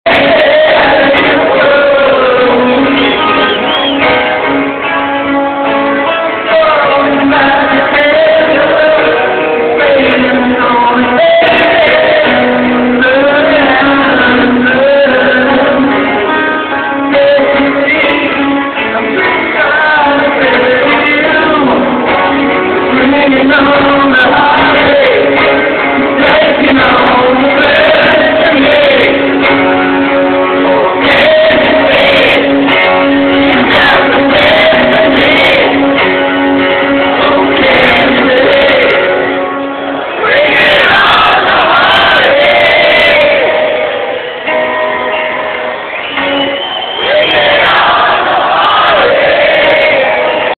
Def Leppard 2009 Jones Beach Concert
Awesome show. The legendary Def Leppard at Jones Beach Theater July 8, 2009